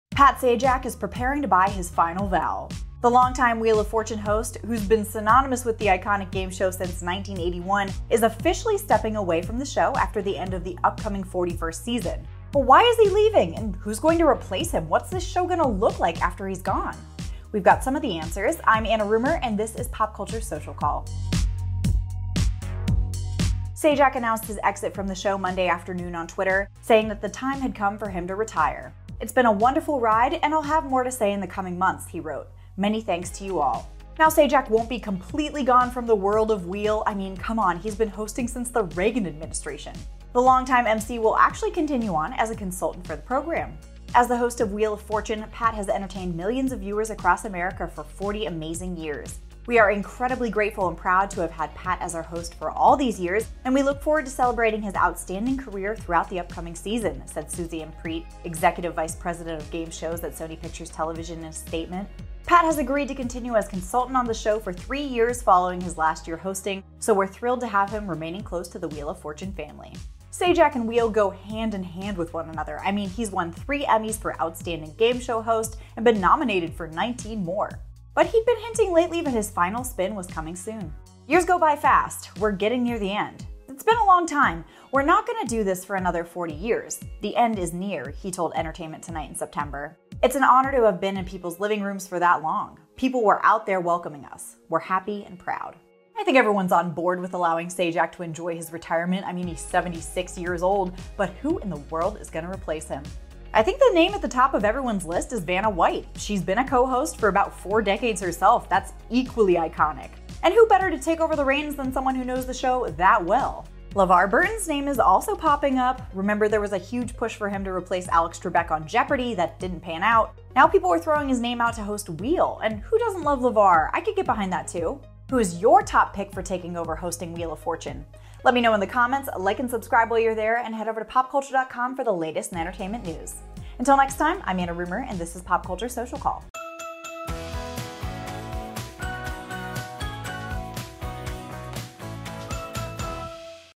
Wheel of Fortune: Pat Sajak Announces Retirement - But Who Will Replace Him?
Wheel of Fortune host Pat Sajak will officially step back from the iconic game show after Season 41 - but who will replace him? PopCulture's Social Call has the latest updates.